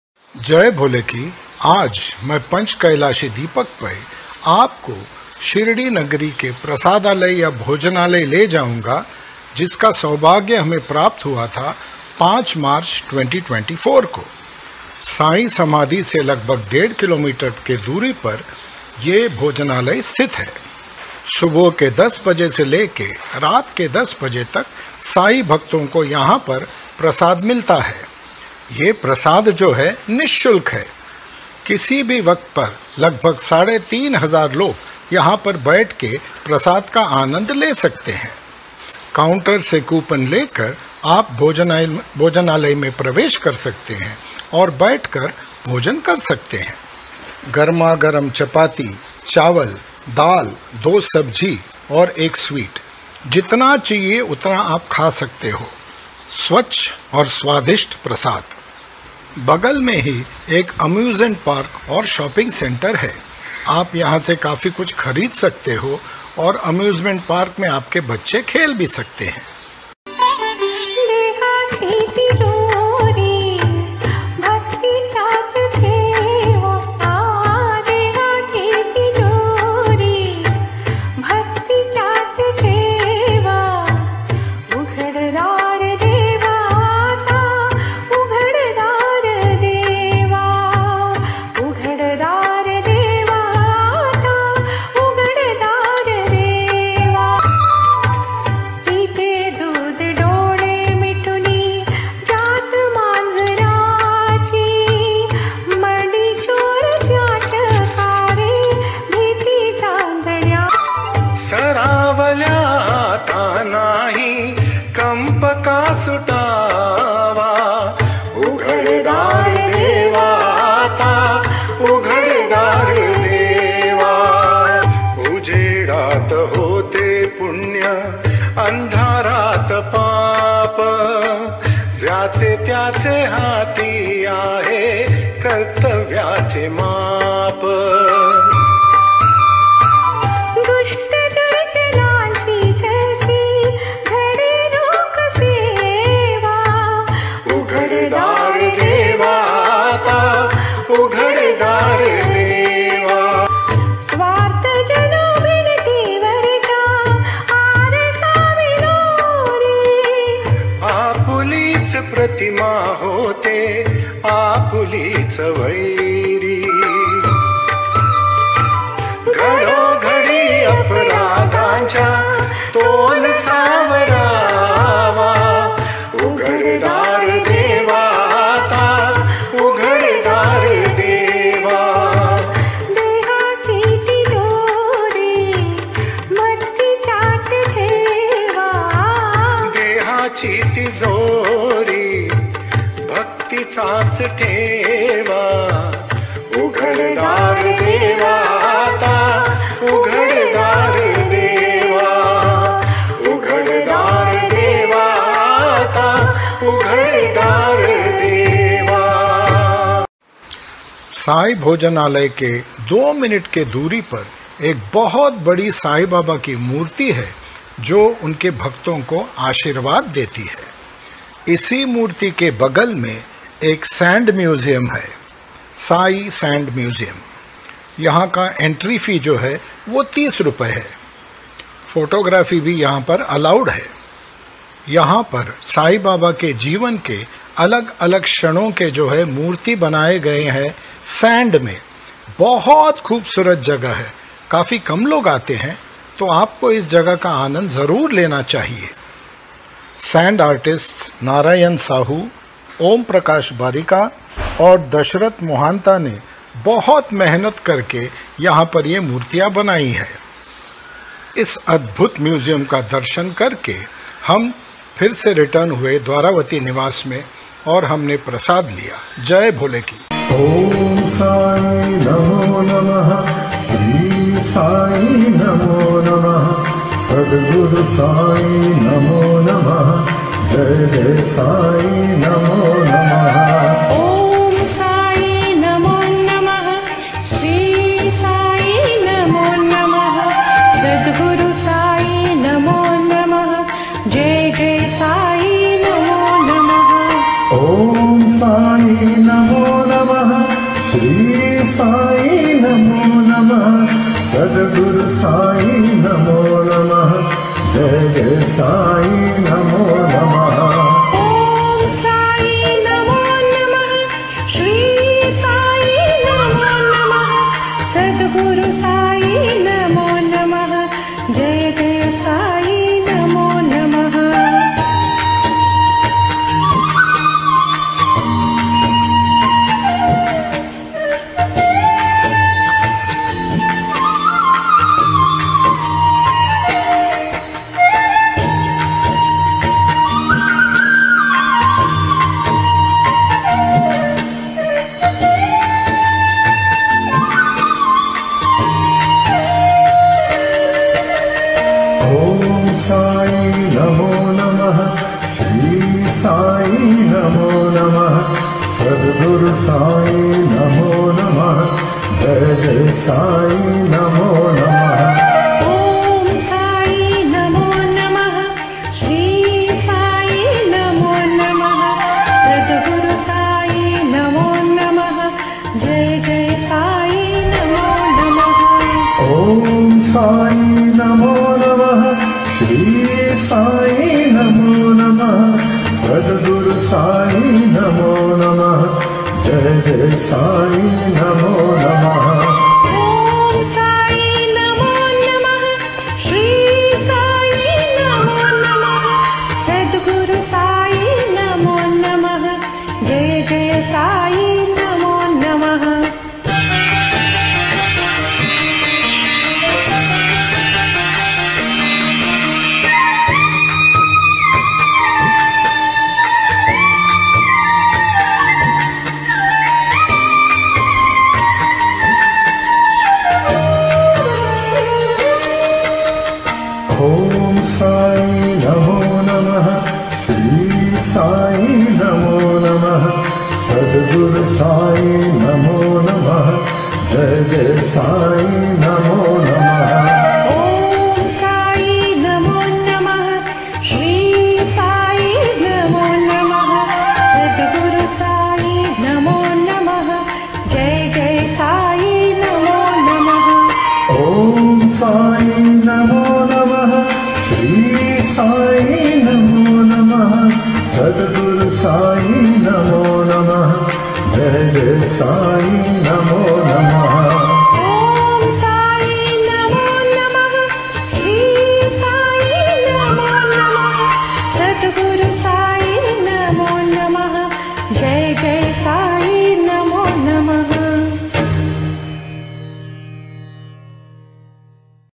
SHRI SAI PRASADALAYA AND SHRI SAI SAND MUSEUM. As on 5th March '24.🙏OM SAI RAM 🙏 JAI BHOLE KI 🙏
🙏 Have a view of the world famous SHRI SAI PRASADALAYA which serves bhojan prasad to one and all free of cost from 10 am to 10 pm every day.
This massive dining area can accommodate 3,500 people at one time. Clean and nutritious food. Next door is the SHRI SAI SAND MUSEUM. A must visit place. Sculptures of sand depicting the various phases of SHRI SAI BABA. Entry fee Rs 30/- only.
🙏OM SAI RAM, 🙏 JAI BHOLE KI 🙏